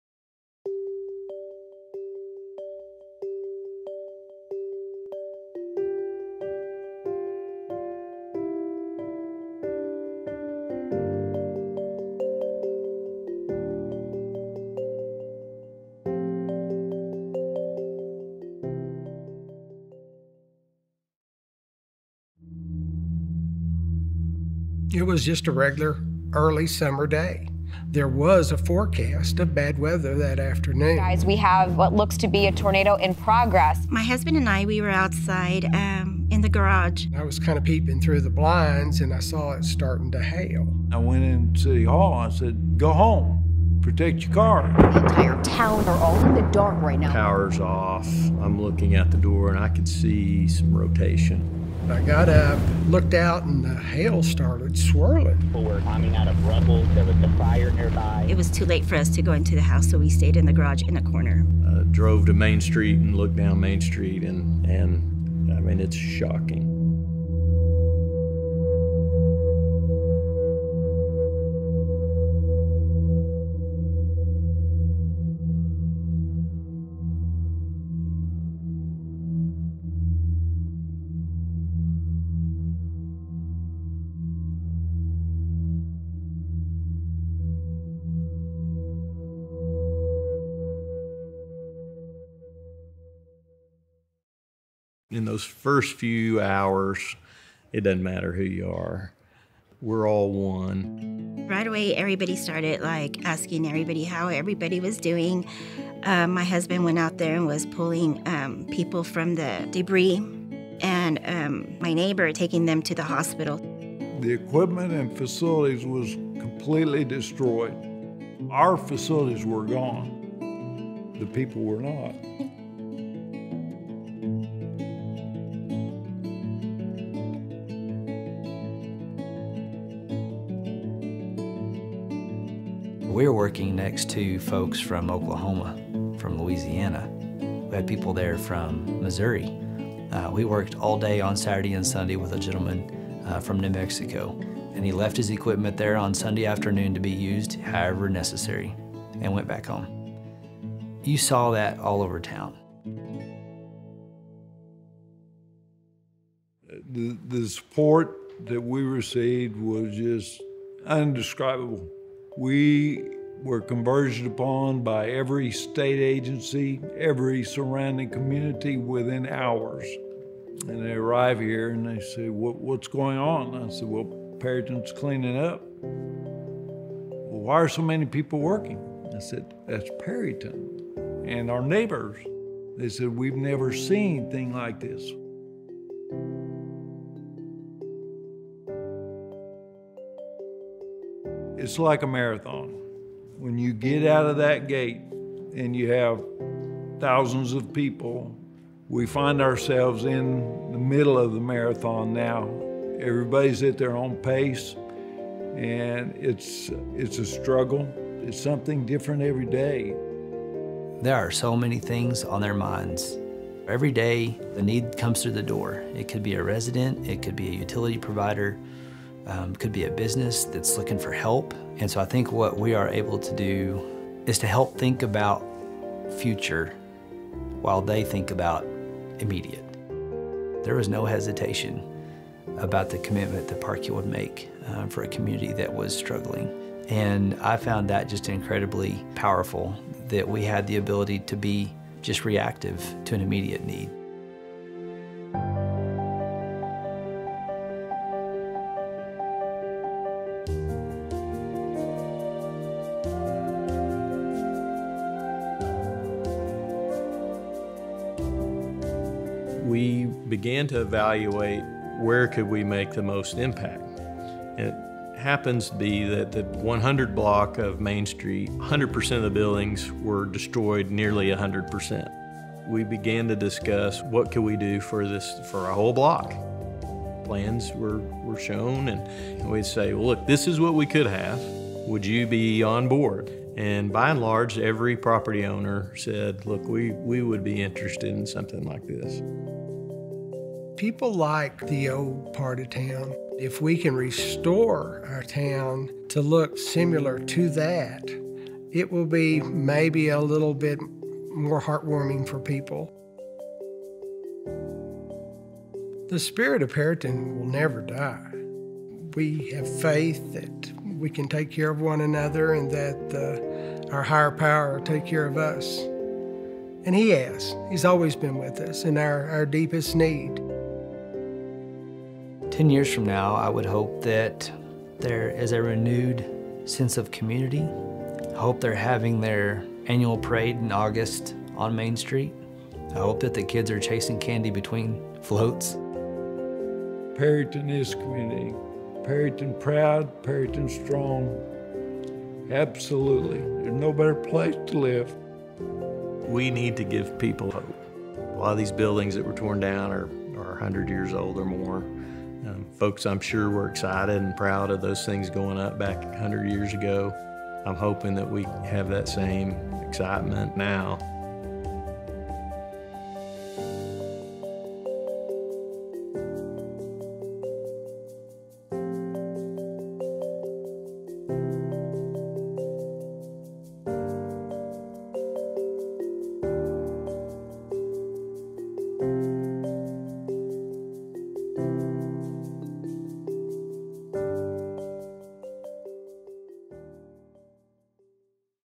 Rebuilding Hope — Perryton, Texas
On June 15th, 2023, tragedy struck the town of Perryton, TX. The need for aid was evident. Through strong community relationships and the services of Parkhill's Building Community Investment, Perryton is all in to restore a once-thriving downtown and enhance its future.

Special Thanks:
David Erickson (Photos)
Matt Hartman (Drone)

Attribution:
ABC 7 Chicago
WFAA-TV
AccuWeather
TwisterChasers
FOX Weather
StormChasingVideo